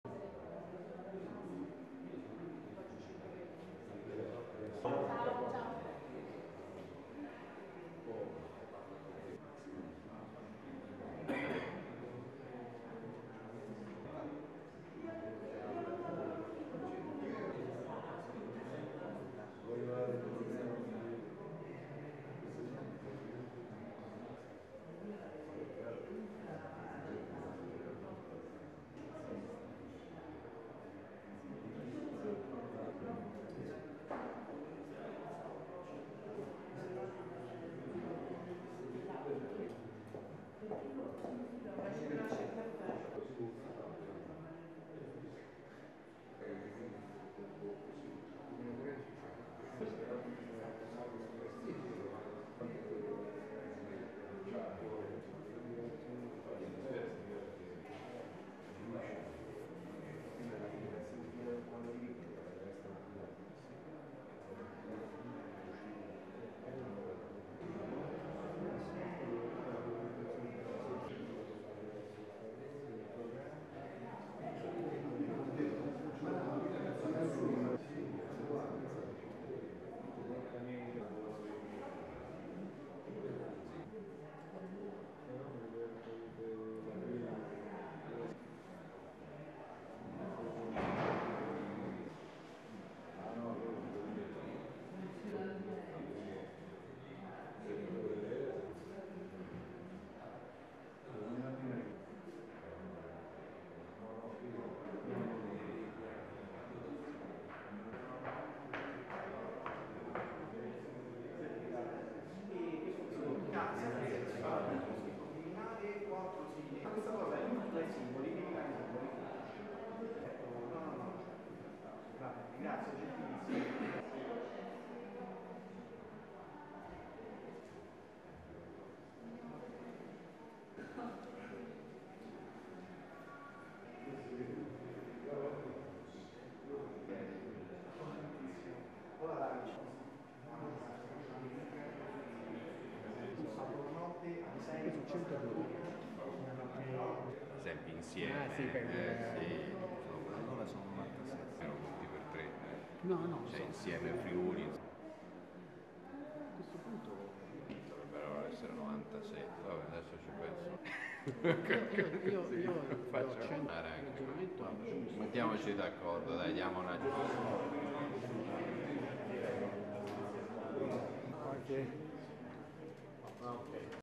Tutti i 98 simboli delle elezioni 2018
(Agenzia Vista) - Roma, 21 Gennaio 2018 - Si chiude la consegna dei simboli per le elezioni politiche del 4 Marzo 2018. Saranno in totale 98 le forze in campo, con alcuni partiti che hanno presentato simboli diversi per le varie circoscrizioni.